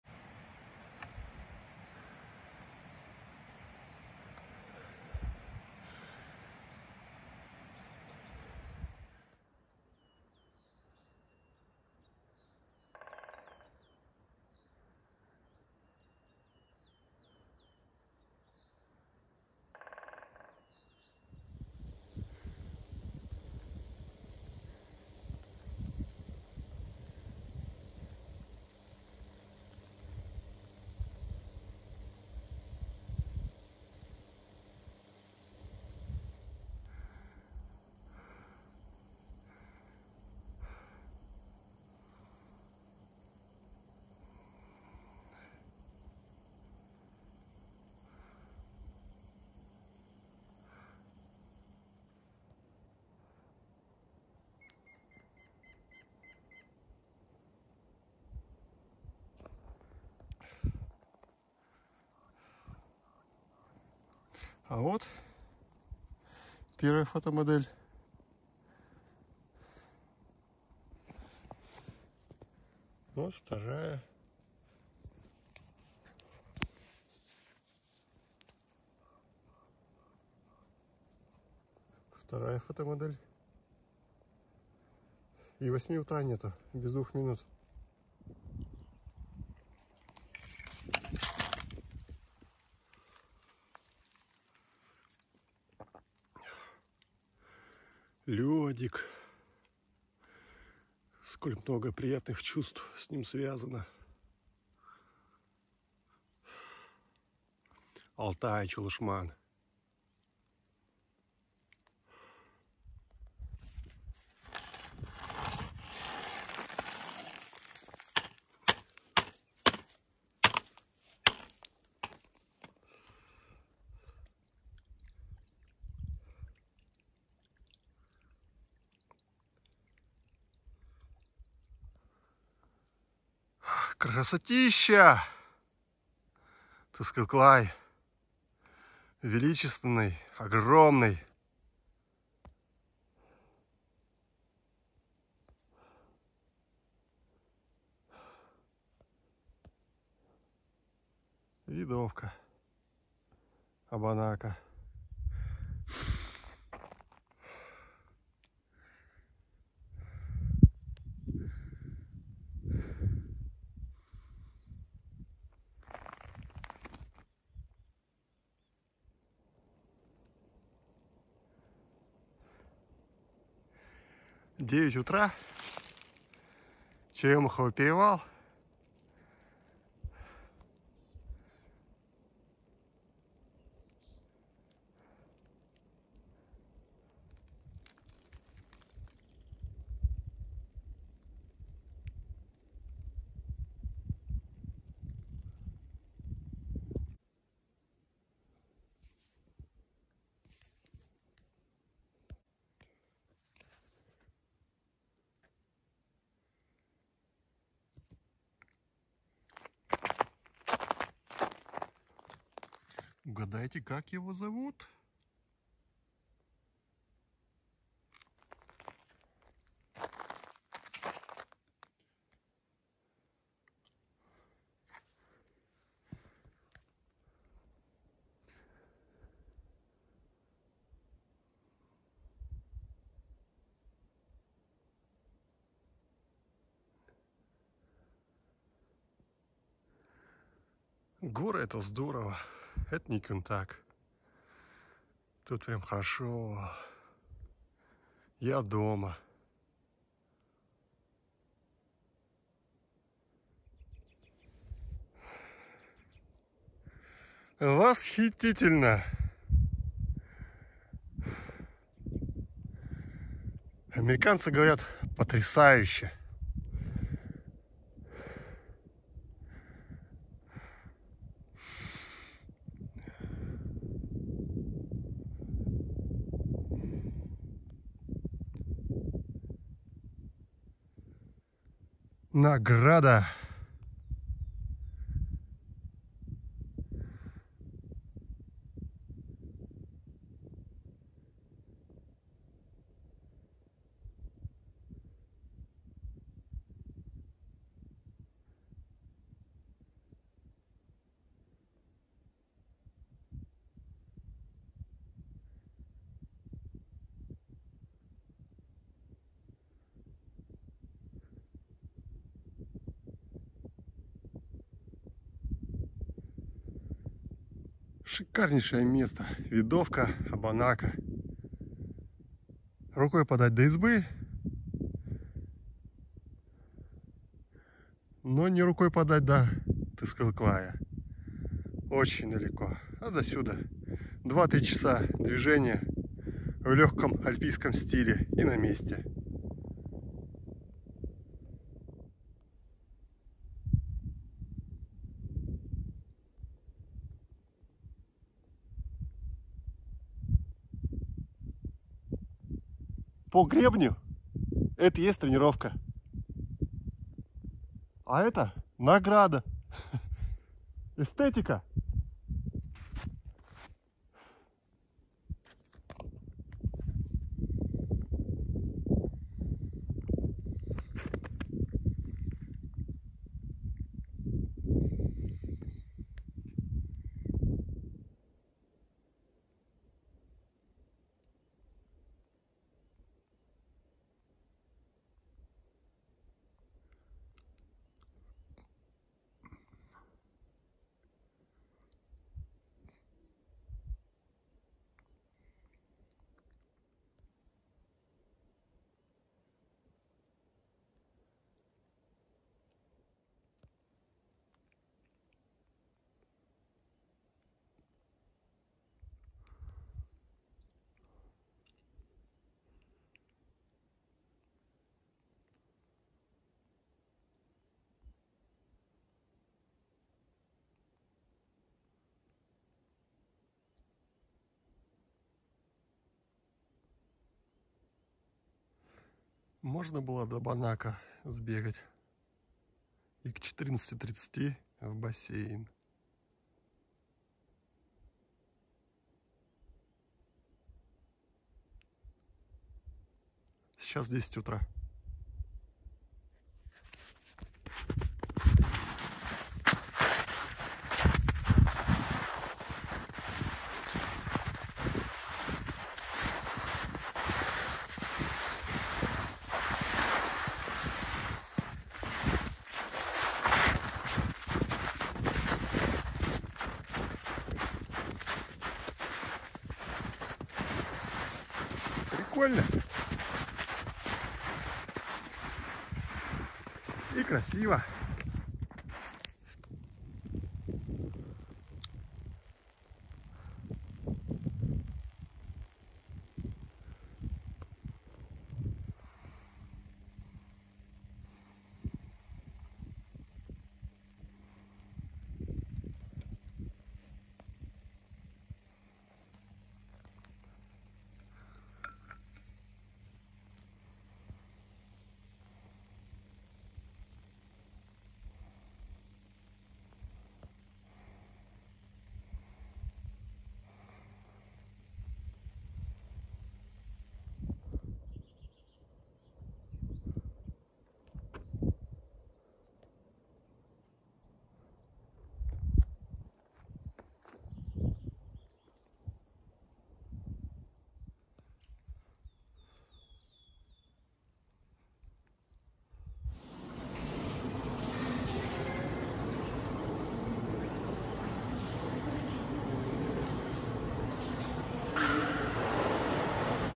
Шикарная тренировка😉
6:15 выезд и понеслось  , перевал в 9 , звонок , на 14:30 бассейн, скоротал время на видовке в 10 и в кафе «Галактика» , наплавался и домой😊